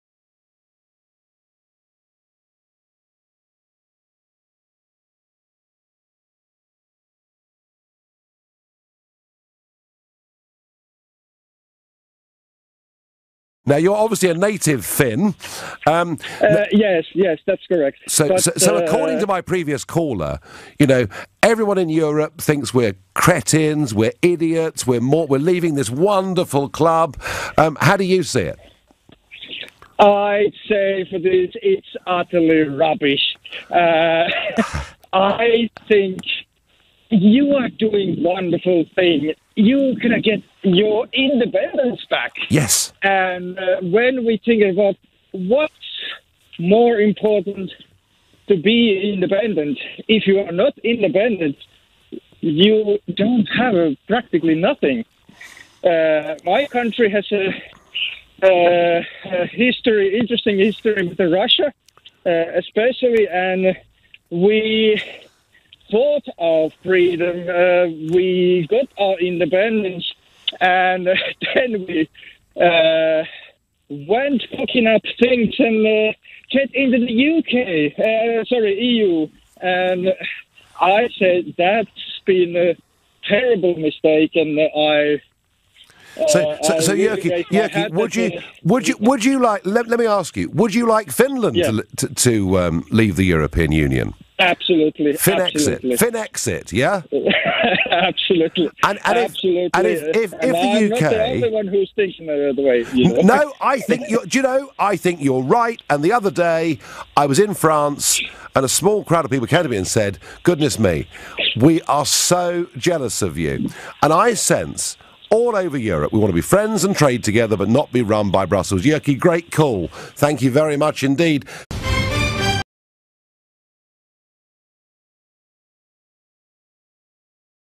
LBC Caller "IM SO JELOUS OF BREXIT" Nigel Farage
Lets check out a Lbc caller who is from Finland and explains how he is so jealous of Brexit and Britain.